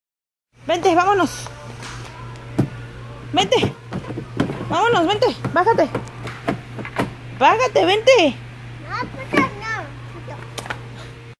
CUANDO ESTA BIEN PEDO Y SE PONE TERCO EL MORRO
Leo se a vuelto un grosero,, pero no se preocupen ya se los estamos quitando!

Snapchat Edna: ednamorales